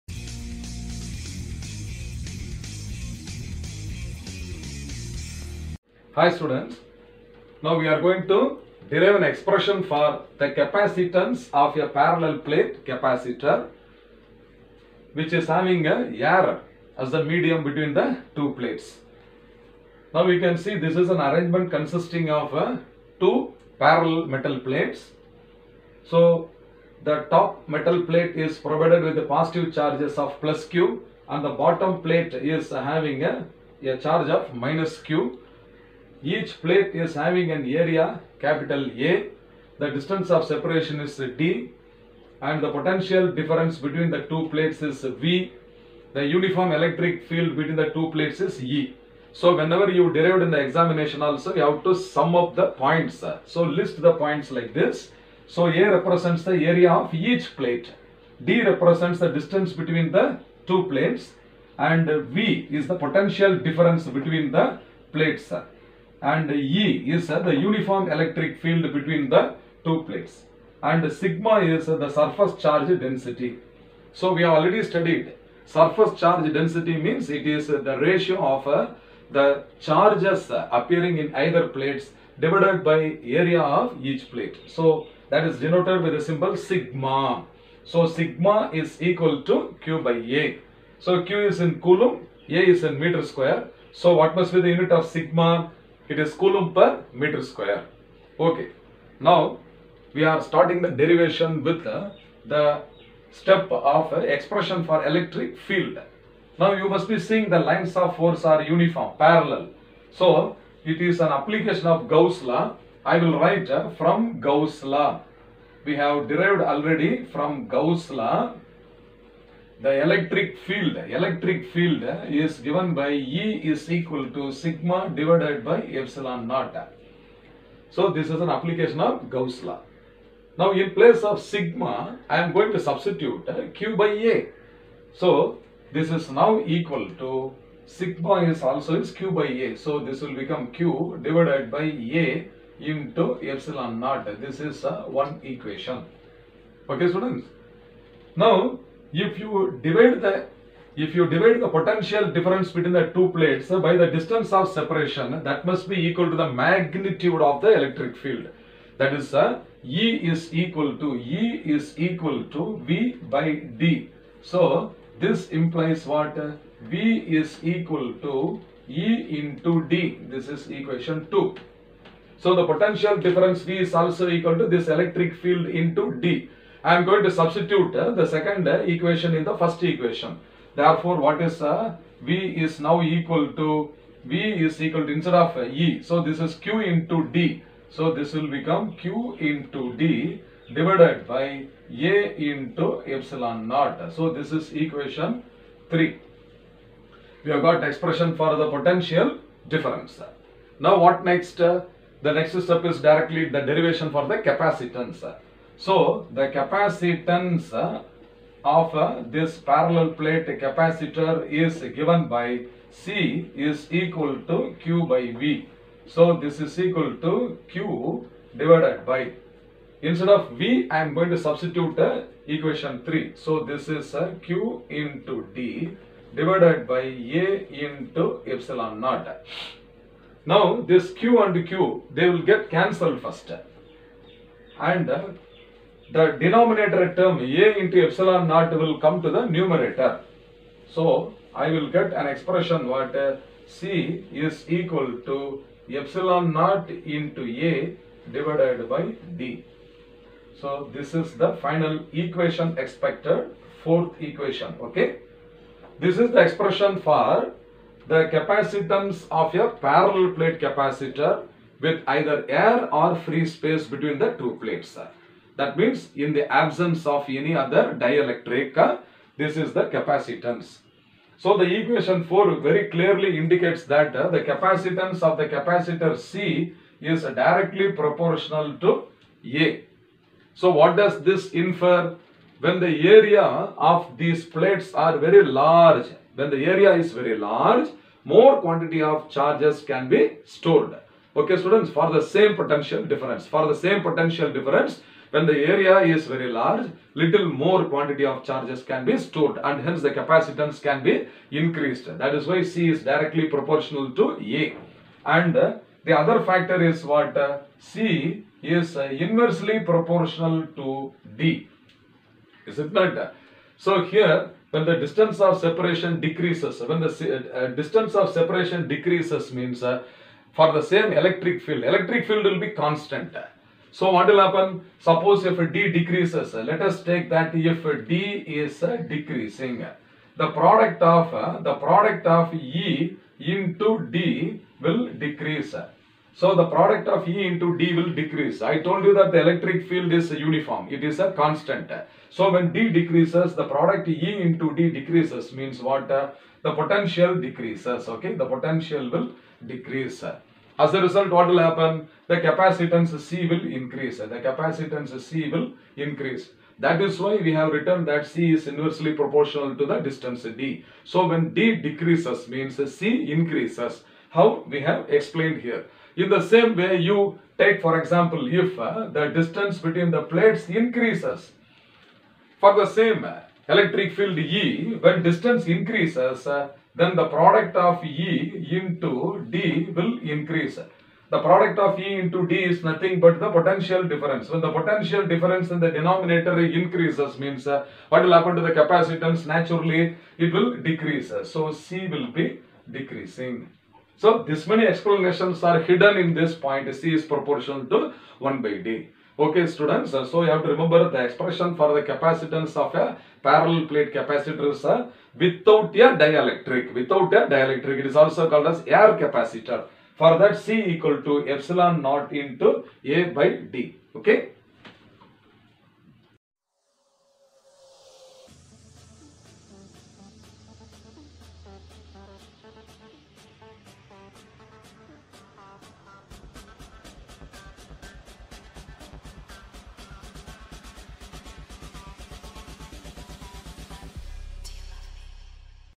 Capacitance of a Parallel plate capacitor - Quick revision | Class 12 Physics | CBSE - TN
It is the derivation for the Capacitance of a Parallel plate capacitor, without dielectric